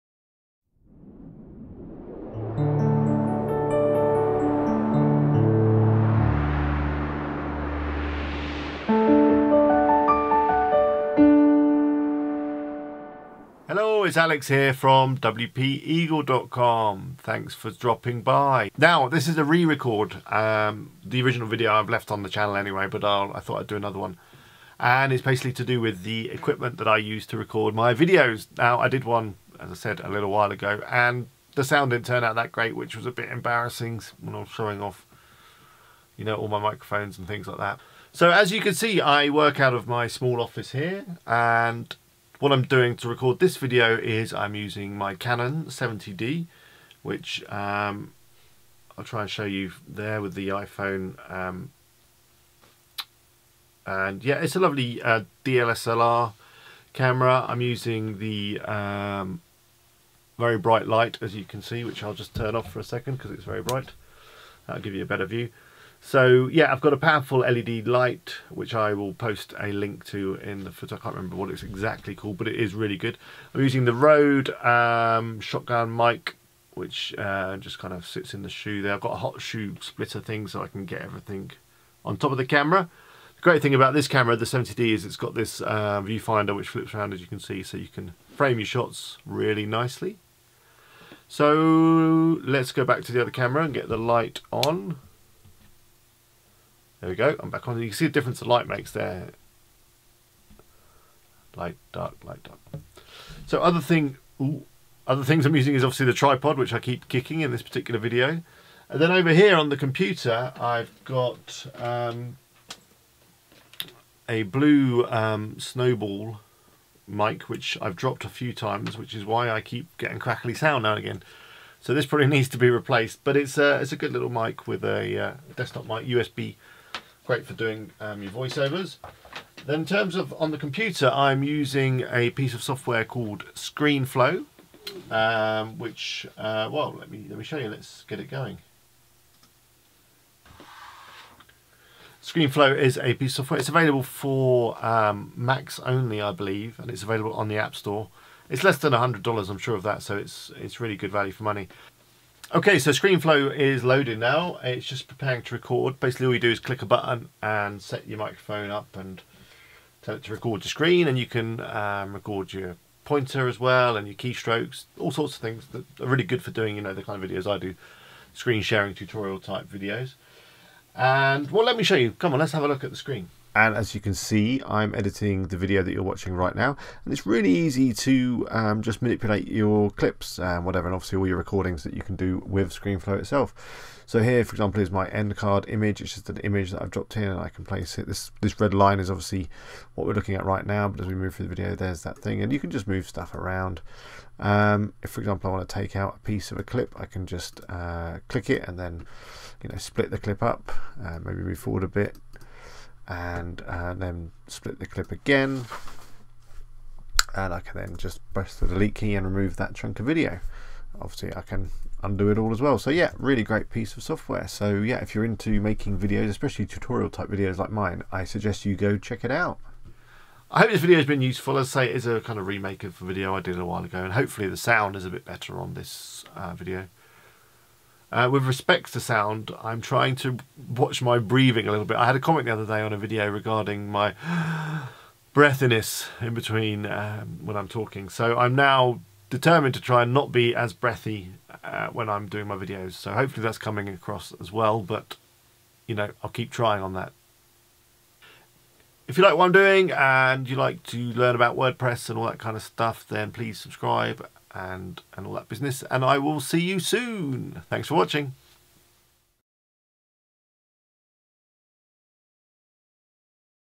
Making YouTube Videos with a Canon & Screenflow
Remake of a previous video on the kit I use to make my youtube stuff. Basically:

Canon 70d
Rode Shotgun Mic
Blue Snowball desktop Mic
Metz Mecalight LED-480